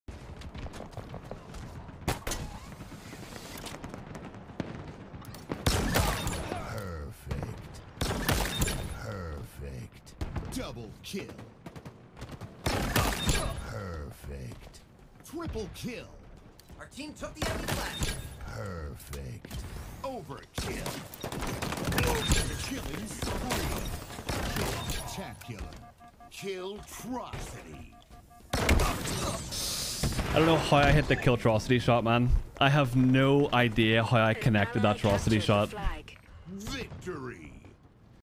SSG Ubernick Gets A Clean KILLTROCITY With The Shock Rifle!!
Ubernick doesn't miss!